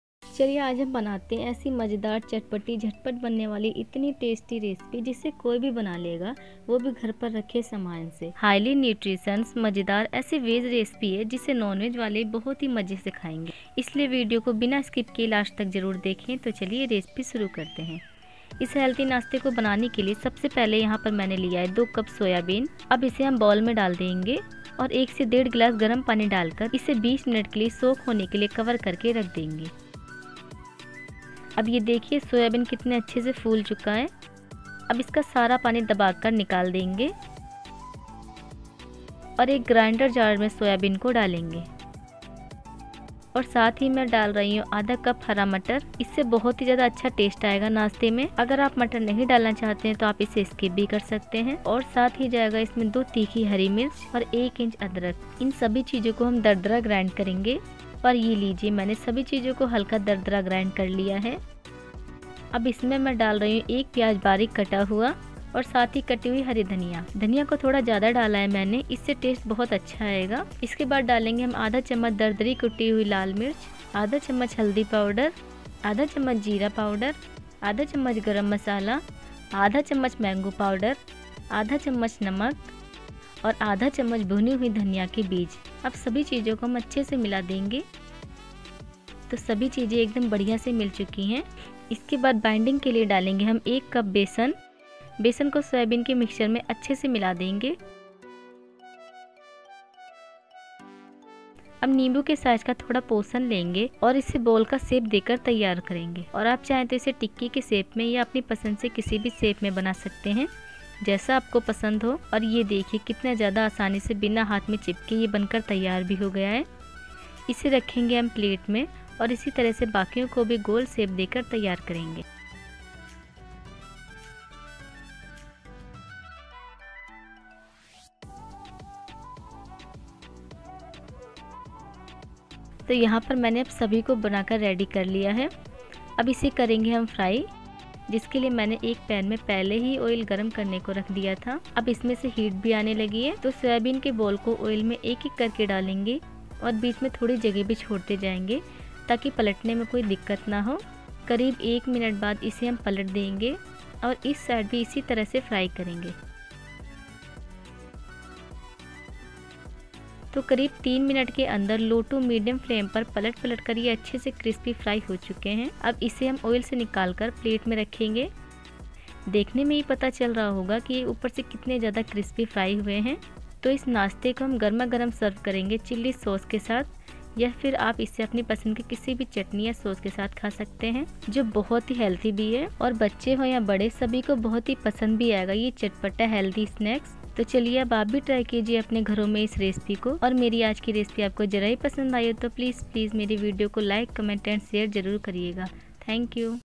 सोयवड़ी से बनाये ऐसा चटपटा नास्ता जिसे देखते ही बनाएंगे और चाय के साथ खायेंगे | Soyabeen snacks
Ingredients👇
1 cup Soaked Soyabeen
1cup Green pea
2 Green chilli
1 inch ginger
1 chopped onions
1/2 cup chopped Green coriander
1/2 tablespoon chilli flakes
1/3 tablespoon turmeric powder
1/3 tablespoon turmeric powder
1/2 tablespoon Cumin powder
1/2 tablespoon Garam masala
1/2 tablespoon Mamgo powder
1 tablespoon Salt
1 tablespoon Coriander seeds
1 cup Gr flour
 to fried oil

ताकि मुझे आपका पुरा स्पोर्ट मिलता रहे और मैं आपके लिए ऐसी ही नई नई रेसिपी लाती रहु..🤗